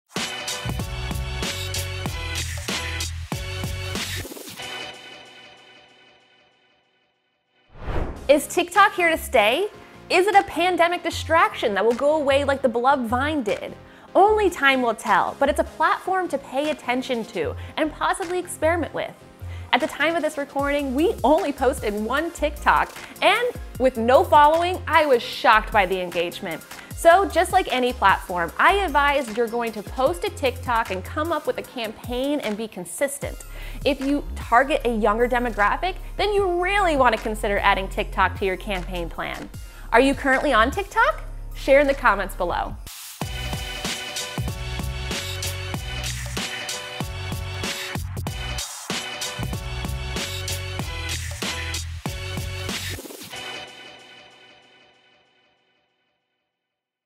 Is TikTok Here To Stay? How To Decide if You Should Market on TikTok
Not sure if you're business should be on TikTok? How to decide if you should market on TikTok, and is TikTok here to stay? Need help coming up with a campaign?

TikTok came on the scene in 2016 however they didn't hit their stride and popularity in the United States till 2020. Currently in October of 2021, TikTok is available in over 150 different markets and has 1.1 billion active users globally.

The age demographics grew dramatically. It started as a place for kids 10-18 years old and has quickly became a place for anyone 65 and below.

Is TikTok here to stay? Is a pandemic distraction that will go away like the beloved Vine did? Only time will tell but it’s a platform to pay attention to and possibly experiment with. At the time of this recording we only posted one TikTok, and with no following I was shocked by the engagement. So, just like any platform I advise if you’re going to post to TikTok, come up with a campaign and be consistent. If you target a younger demographic, then you really want to consider adding TikTok to your campaign plan.